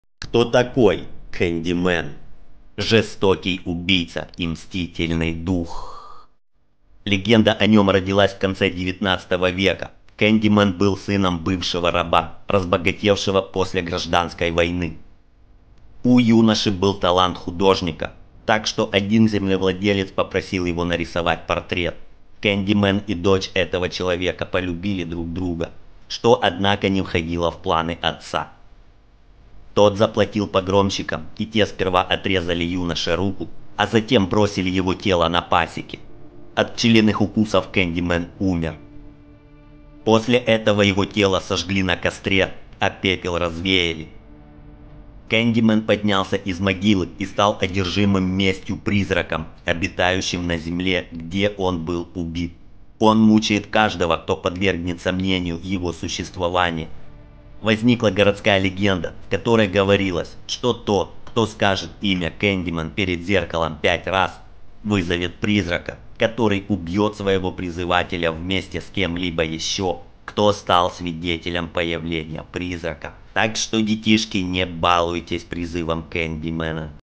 Кто такой Кэндимэн | Candyman | Страшилки из Тик Тока
😈 Всем БУ! В этом видео вы узнаете историю Кэндимена.